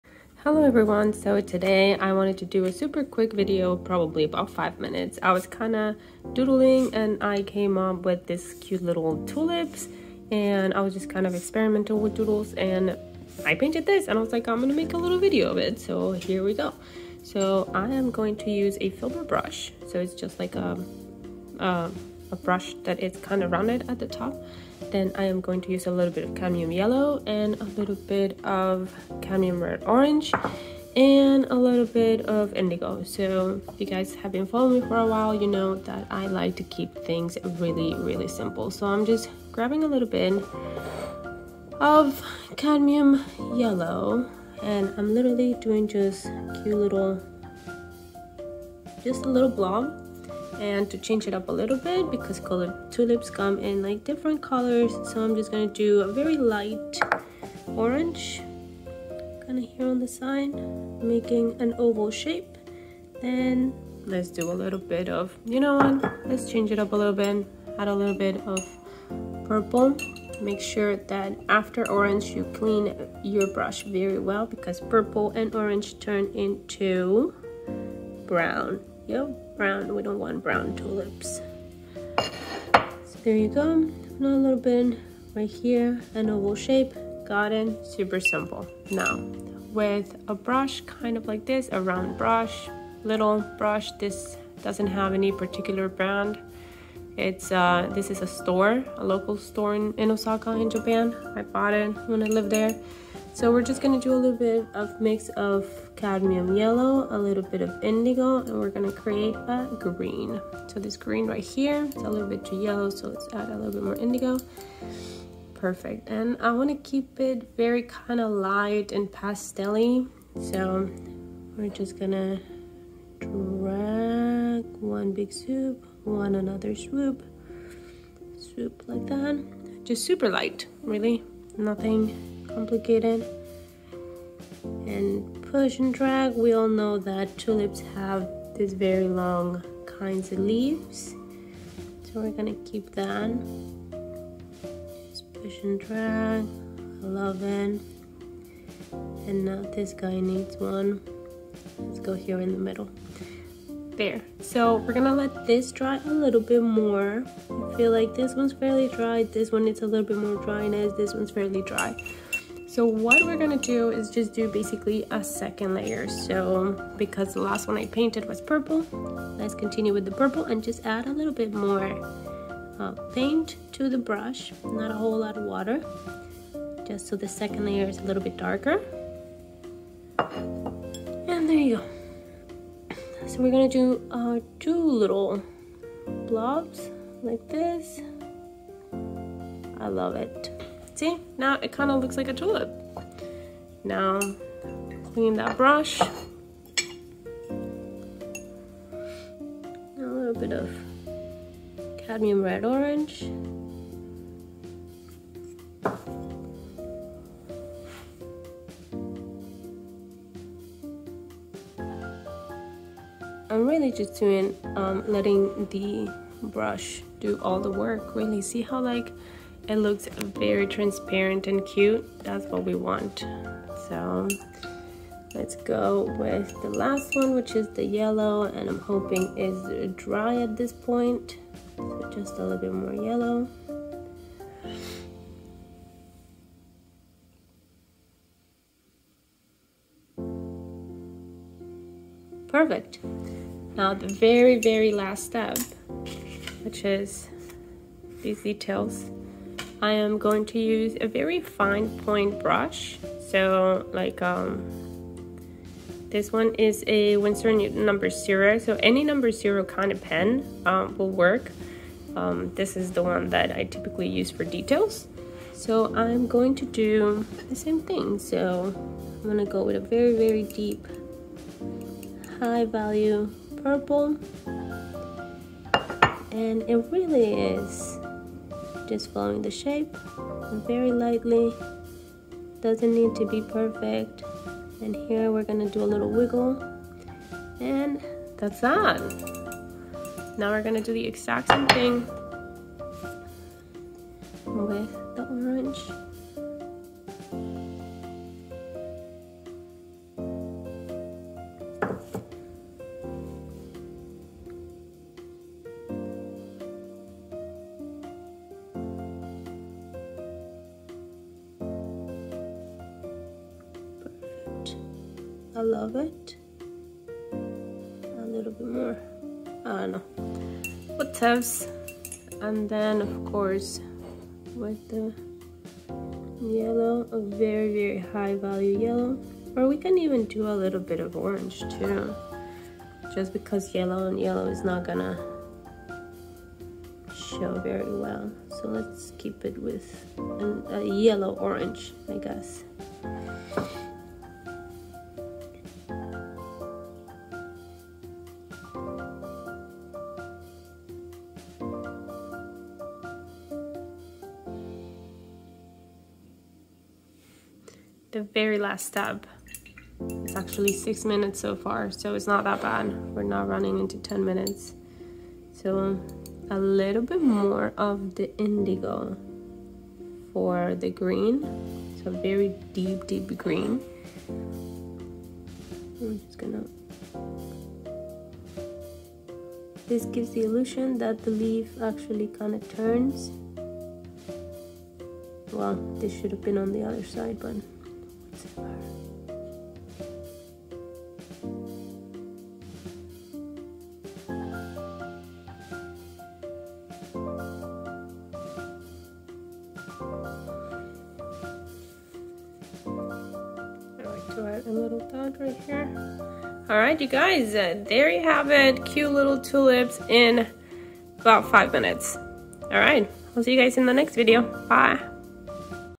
Painting Watercolor Tulips - Watercolor painting for beginners - Abstract Watercolor Flower
Hello again Beautiful friends! Hope you enjoy this little tutorial.
In this video, I'll show you how I Painted these Watercolor Tulips in a step by step tutorial, so we can start painting watercolor tulips together. This is a great Painting Watercolor Tulips project for beginners who want to learn how to paint abstract watercolor flowers.

We'll be using simple watercolor techniques to create beautiful abstract flower paintings. This is a great project for anyone who wants to learn how to paint watercolor flowers, and it's a great way to get started with watercolor painting.